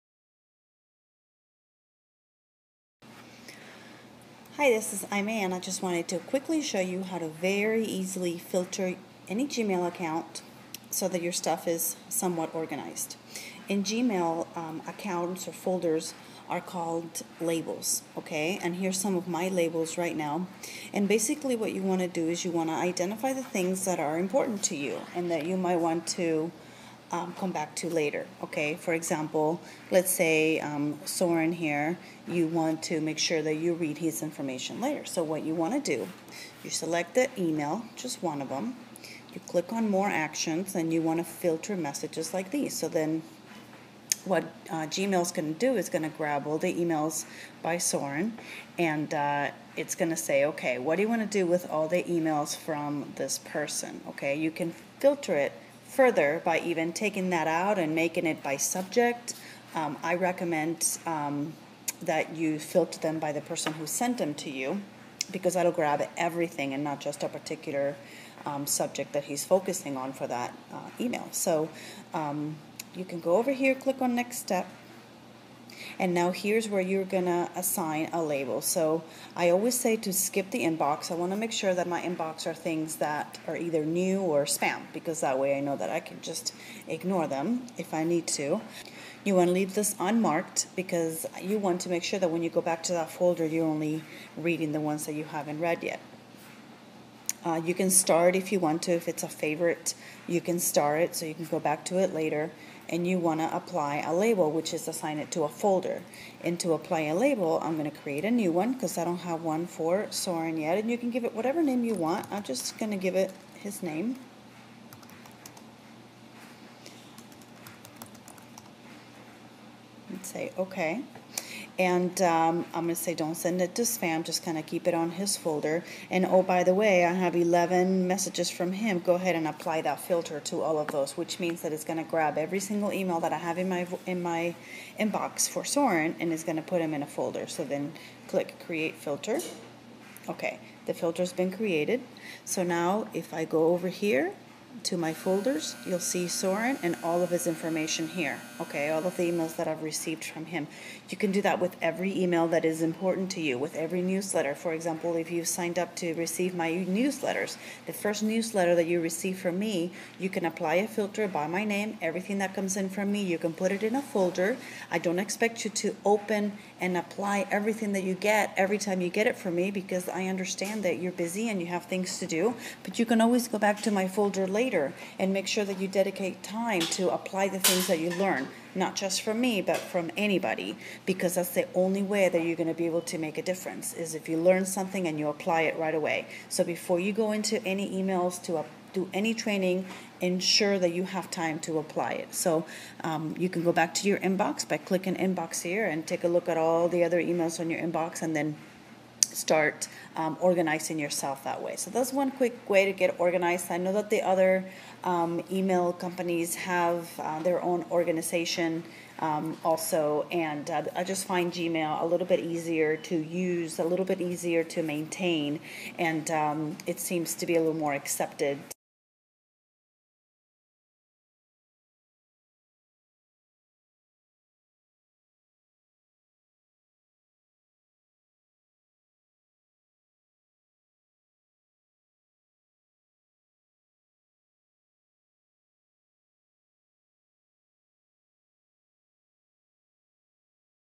Getting Organized With A Gmail Account: Gmail Folders, Labels
One of the most important elements of "sanity" is organization. That truly is key if you are doing anything online. Have the newsletters that you care about neatly placed in their own folders quickly, easily and automatically so that you can go back to them later and "learn and apply" what you need! It's really a zinch with applications like gmail.